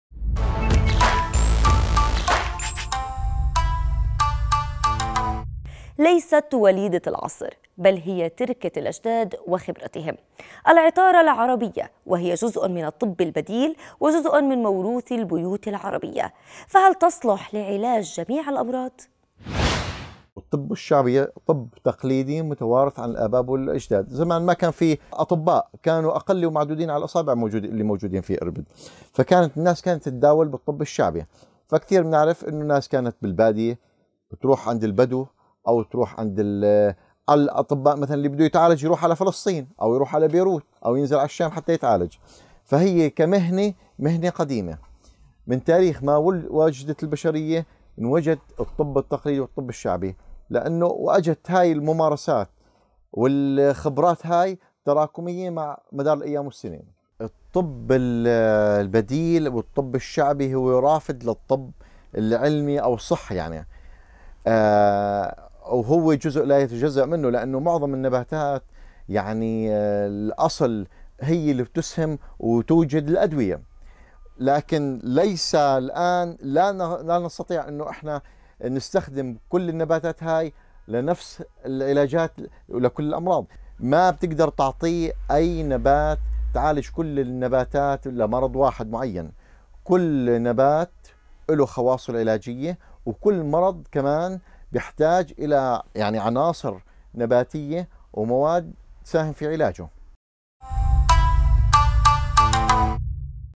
هل تصلح العطارة لعلاج جميع الأمراض؟ - أخبار الدار
ليست وليدة العصر بل هي تركة الأجداد وخبرتهم .. العطارة العربية .. وهي جزء من الطب البديل وجزء من موروث البيوت العربية. فهل تصلح لعلاج جميع الأمراض ؟
تقرير حمزة بيبرس